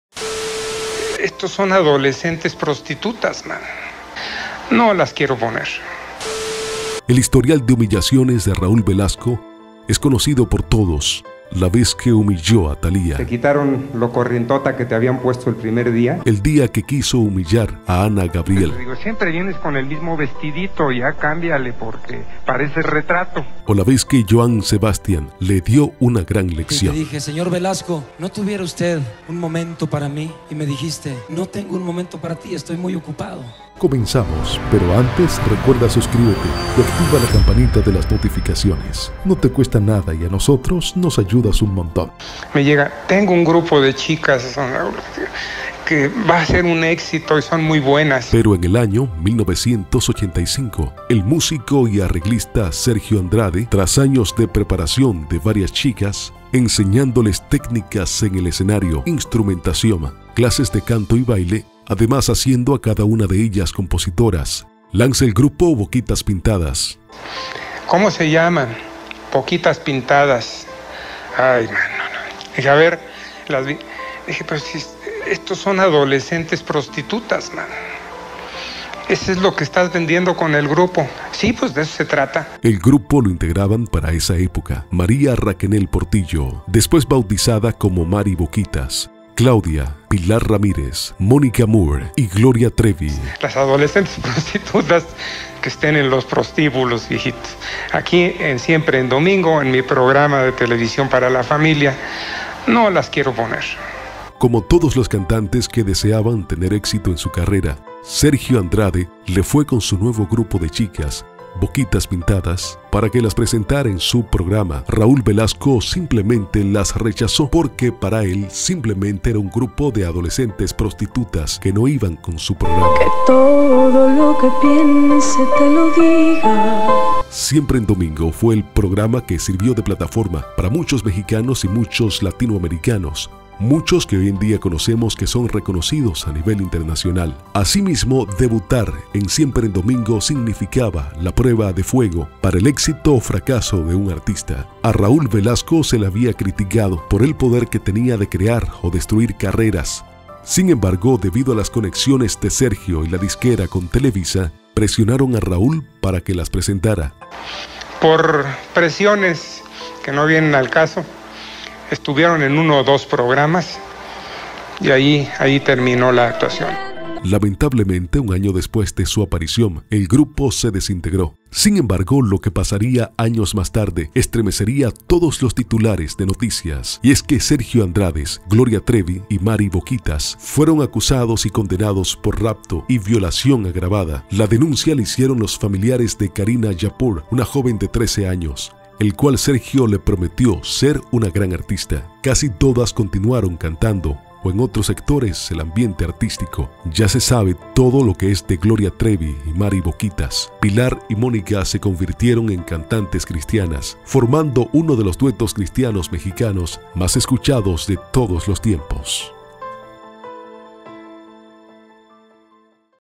EL DIA QUE UN PRESENTATOR LLAMO PR0ST1TUT EN TV A UNAS CANTANTES Y SE LAMENTO
Biografia boquitas pintadas